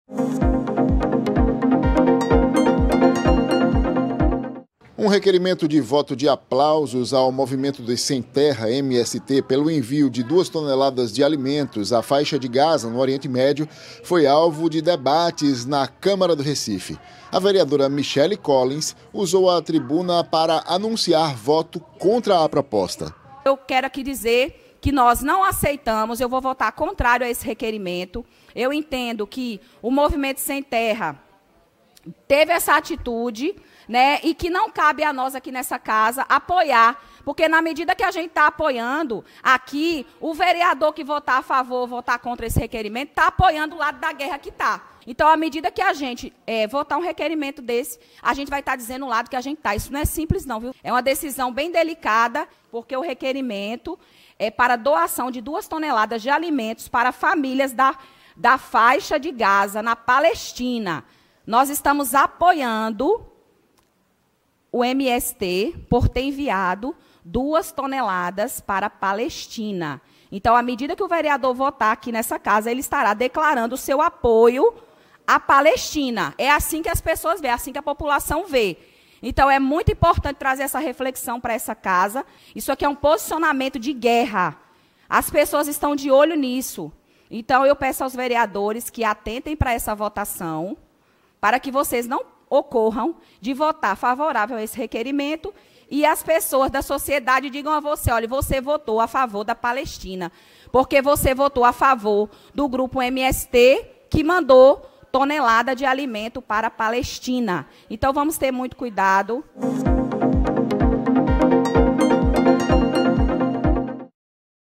Michele Collins anuncia posicionamento contra voto de aplausos ao MST
Um requerimento de voto de aplausos ao Movimento dos Trabalhadores Rurais Sem Terra, MST, pelo envio de duas toneladas de alimentos à Faixa de Gaza, no oriente médio, foi alvo de debates na Câmara do Recife. A vereadora Michele Collins usou a tribuna para anunciar voto contrário à proposta.